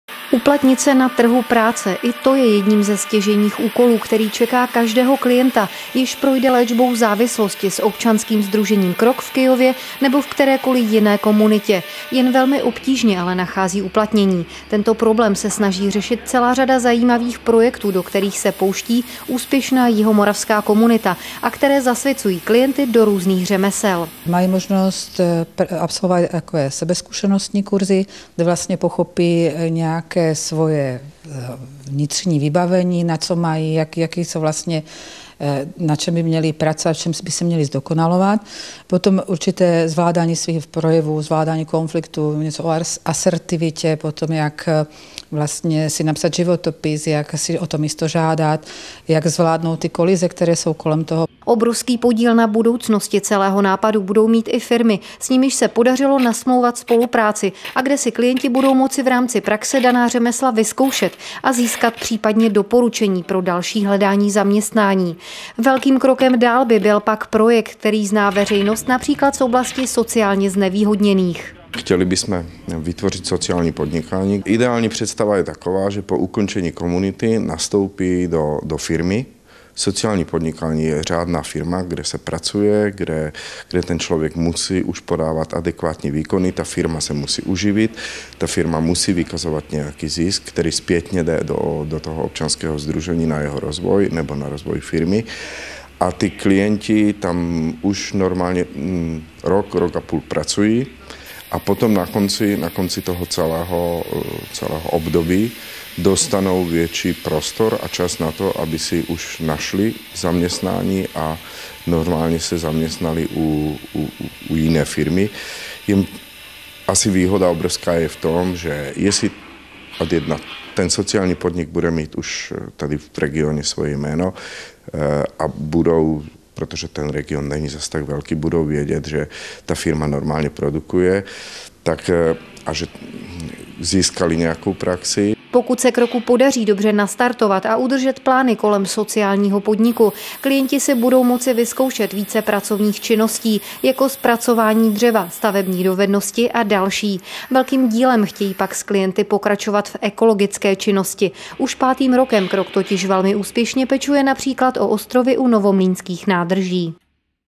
TV Slovácko: Kyjov - Krok pomáhá vyléčeným hledat zaměstnání
Sehnat odpovídající práci je často velmi obtížné. Své o tom ví i bývalí klienti komunit pro léčbu závislostí. Právě jim mají pomoci začlenit se dlouhodobě připravované projekty OS Krok, které se v nich zaměřilo na návaznou pomoc.